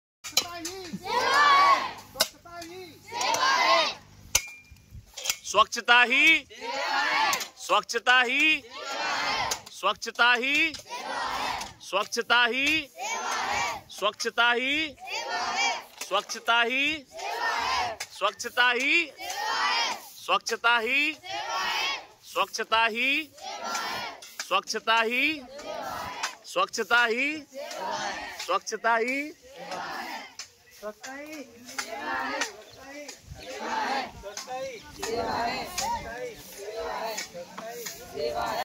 October 2, 2024
गाँधी जयन्ती एवं शास्त्री जयन्ती पर NSS के संयोजन से विद्यालय में स्वच्छता अभियान चलाया गया